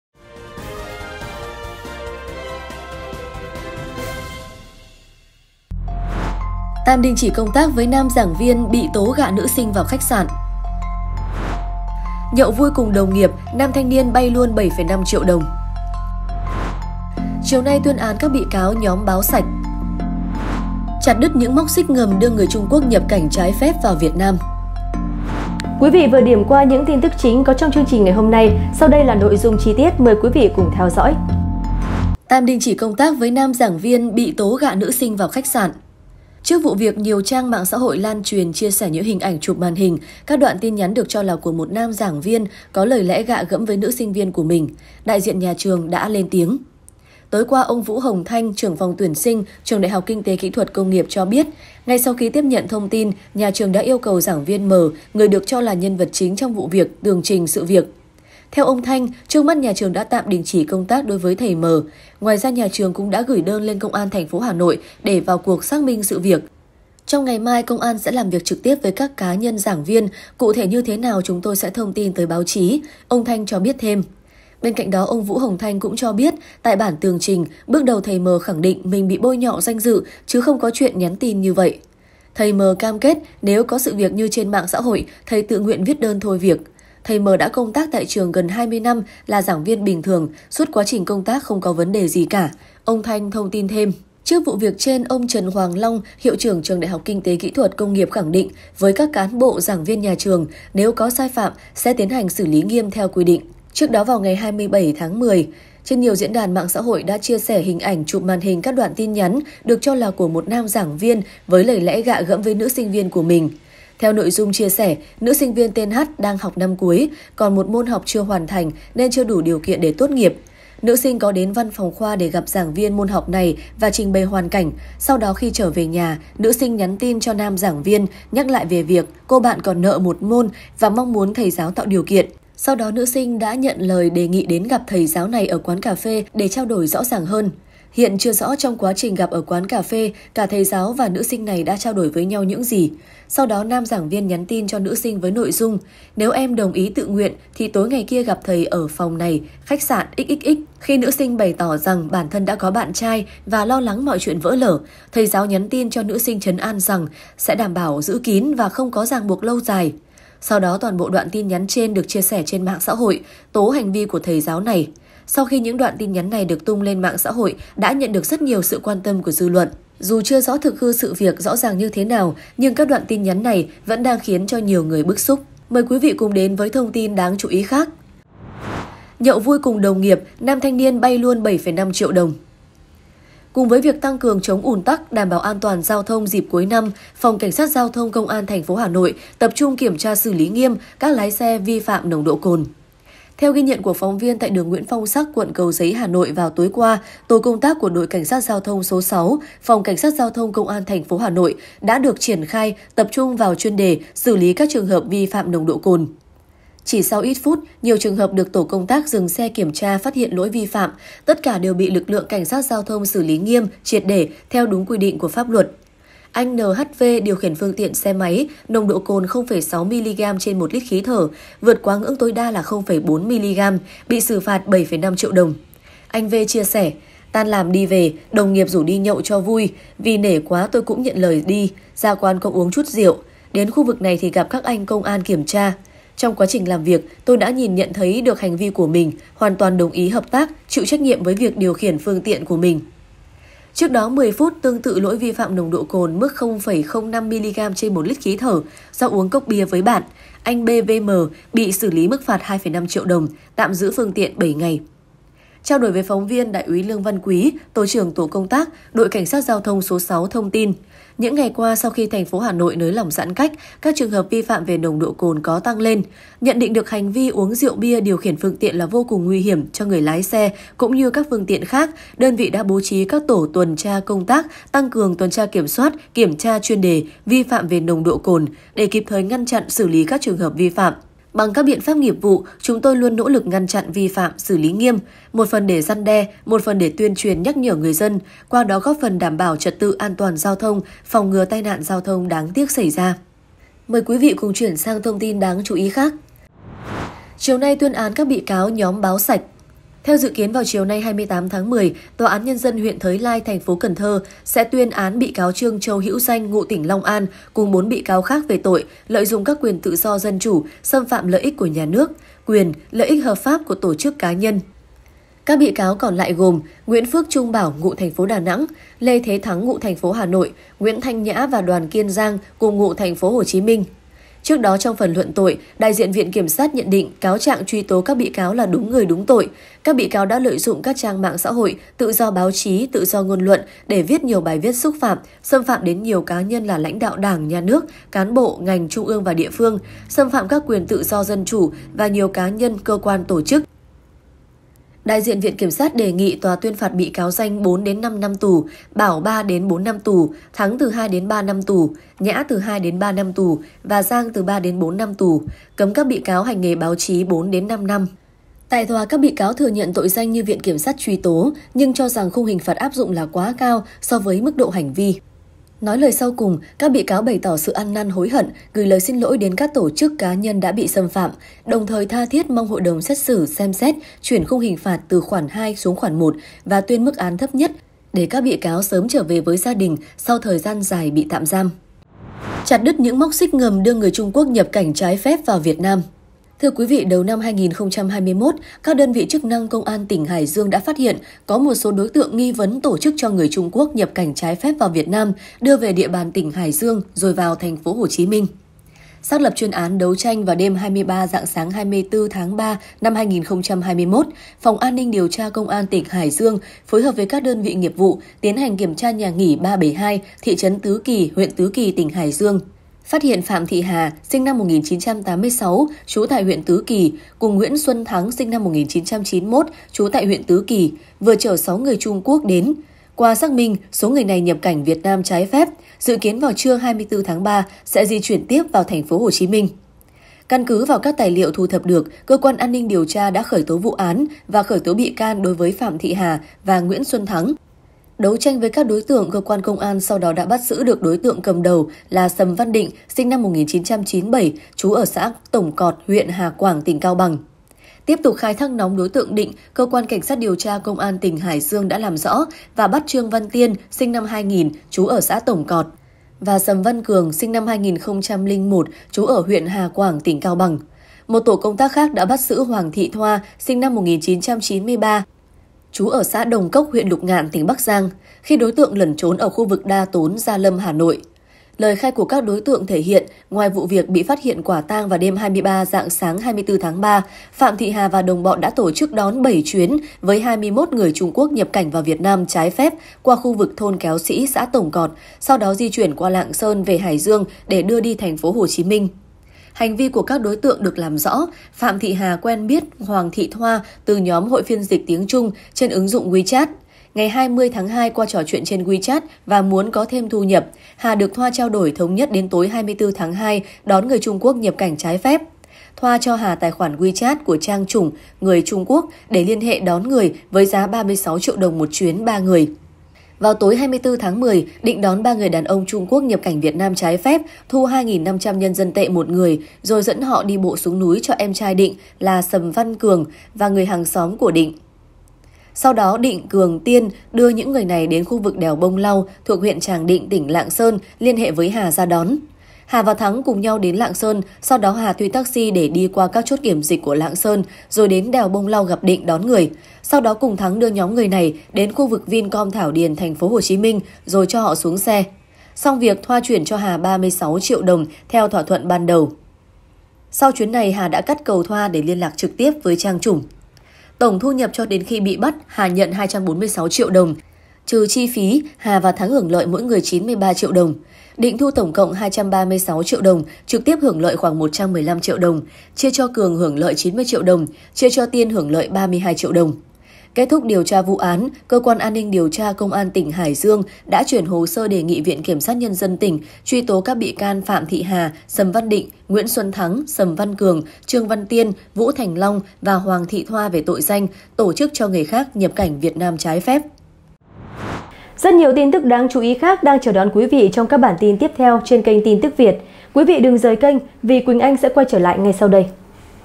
Chặt những MÓC XÍCH NGẦM đưa người Trung Quốc nhập cảnh trái phép vào Việt Nam |TIN AN NINH CỰC NÓNG
🔥 Tin An Ninh Cực Nóng | Thời Sự Mới Nhất Và Chính Xác Nhất | Tin tuc 24h moi nhat hom nay cập nhật bởi @TinTucVietOfficial
Các tin tức có trong [ 🔥 TIN AN NINH CỰC NÓNG ] :

00:31 Ngày 17/9: Có 11.521 ca mắc COVID-19, trong đó
TP.HCM và Bình Dương đã gần 10.000 ca
02:58 Nhậu 'vui' cùng đồng nghiệp, nam thanh niên 'bay' luôn 7,5 triệu đồng
04:56 Chiều nay, tuyên án các bị cáo nhóm 'Báo Sạch'
06:22 Chặt những móc xích ngầm đưa người Trung Quốc
nhập cảnh trái phép vào Việt Nam